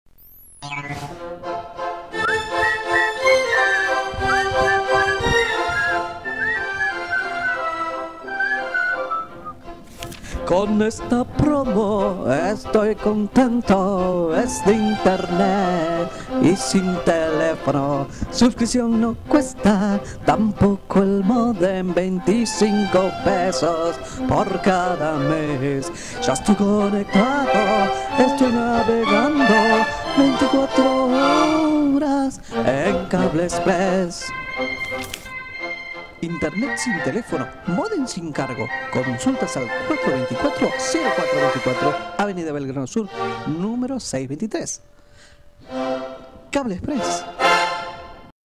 FINAL PARA TV JUANCITO
Una publicidad que hice para Cable express. Es un trabajo con fotografías y dibujos. La voz la puso un compañero.